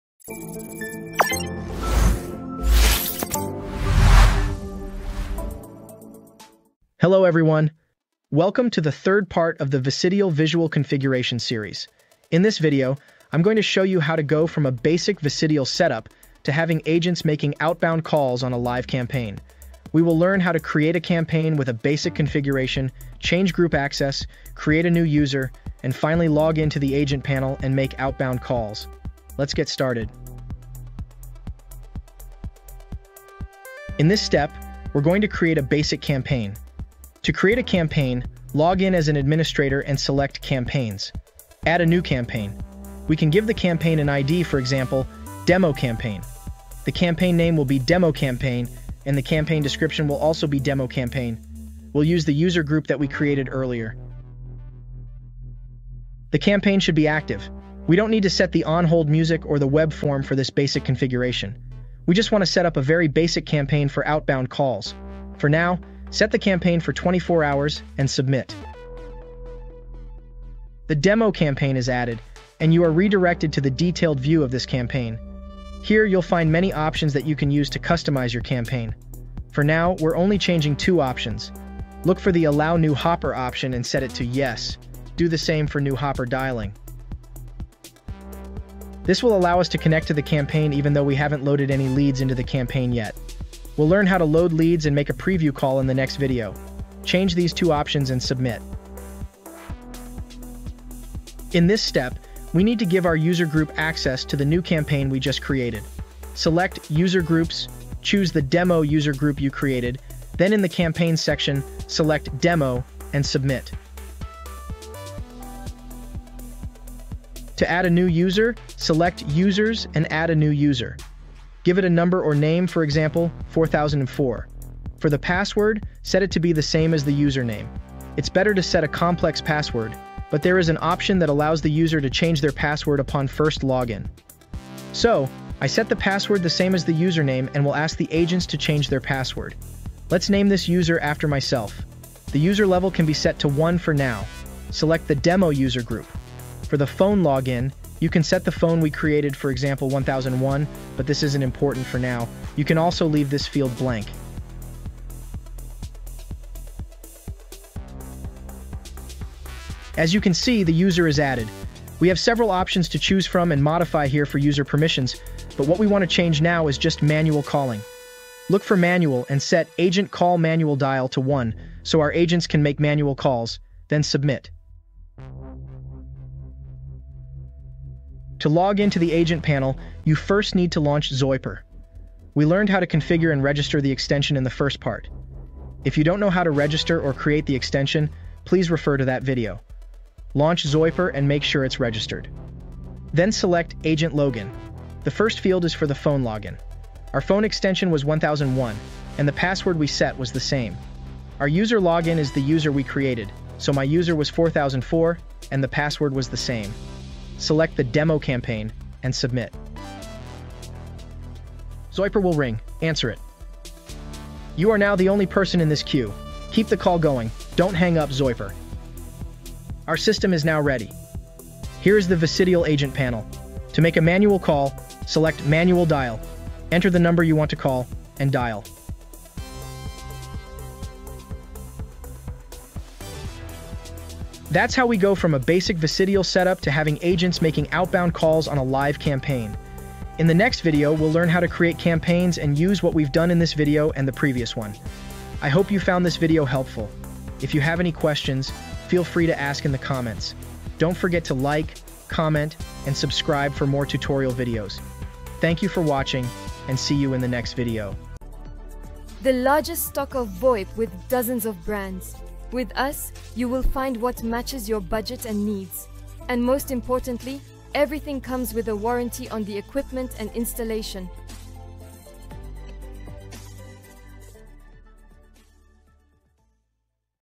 EP3 How to Create and Edit a Manual Dialing Campaign in Vicidial: Step-by-Step Guide
Want to master creating and editing manual dialing campaigns in Vicidial? Watch our step-by-step guide to learn how to create and upload a call list effectively.

This video offers a detailed simulation of agents making manual calls, helping you refine your campaign management skills.